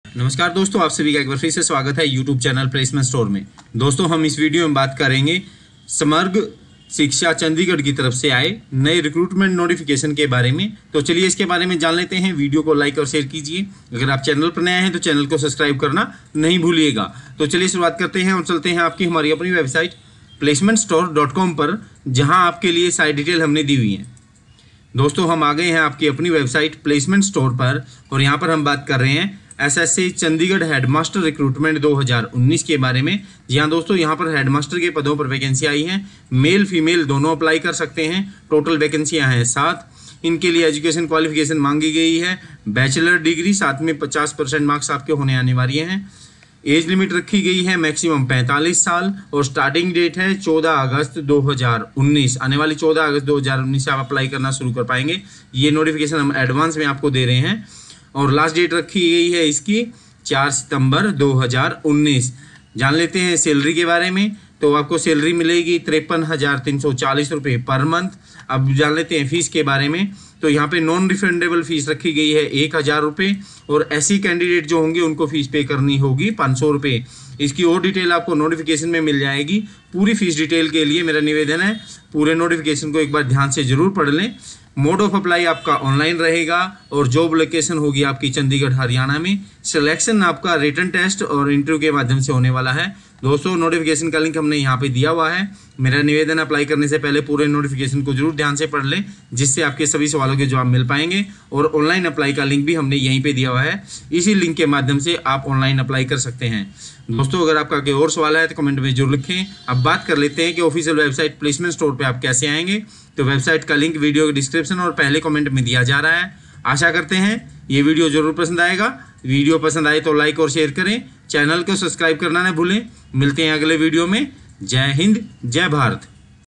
SSA Chandigarh Headmaster Recruitment 2019 ¦ Apply Online 07 Post
SSA C Headmaster Jobs 2019: Samagra Shiksha Chandigarh has issued a latest notification for the recruitment of Headmaster/ Headmistress Vacancy at 07 posts. Interested candidates can apply by 4 September 2019. Other details of SSA Chandigarh Recruitment 2019, SSA Recruitment 2019
►Source Link ✔
►Android Apps ✔
Placement Store - Android Apps on Google Play

► Hi, Welcome to official The Placement Store™ Youtube channel.